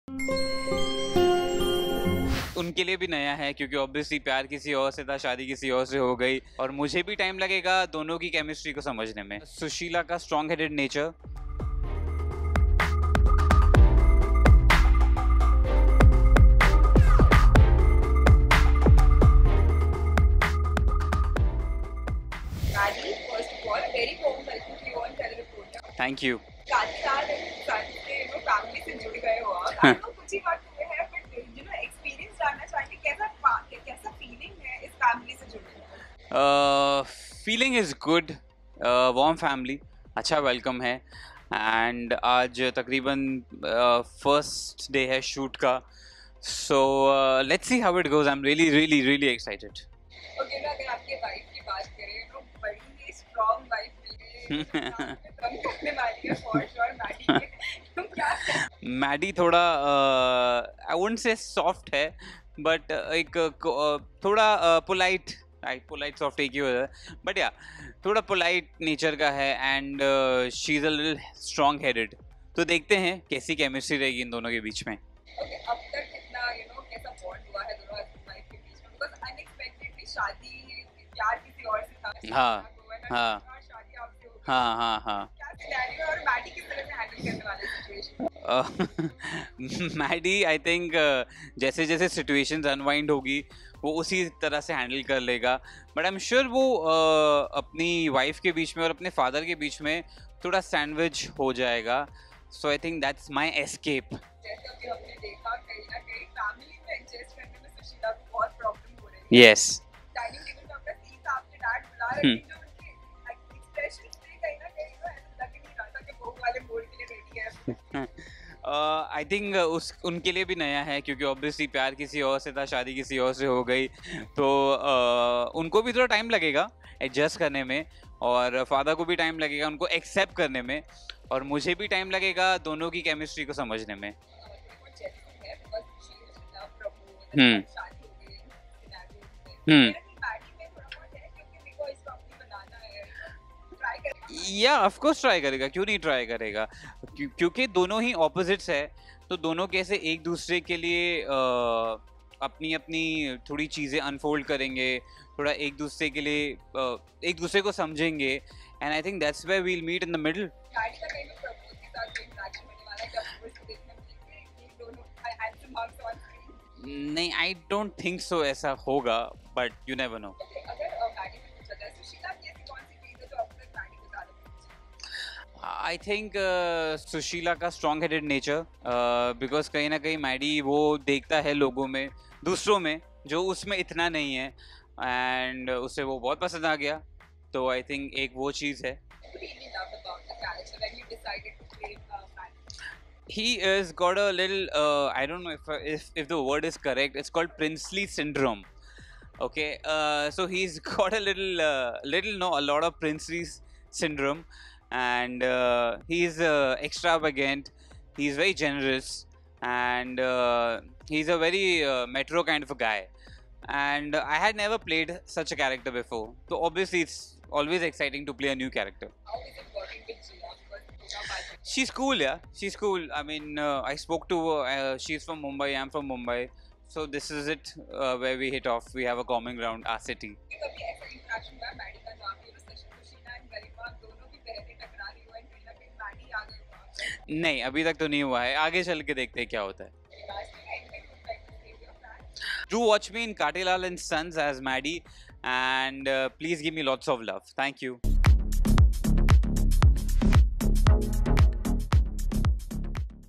Kaatelal & Sons Vinay Rohra Interview: On Playing Maddy, Marrying Susheela, Dr Pramod & Lot More
Kaatelal & Sons New Entry Vinay Rohra aka Maddy Who Will Be The New Love Interest Of Susheela In The Show Exclusively Got In Conversation With Telly Reporter. Where He Spoke About His Character Maddy Aka Madhav. Susheela aka Jiya Shankar Playing Sanskari Bahu. Maddy Interaction With Dr. Pramod aka Paras Arora and Lot More. Watch This Video For More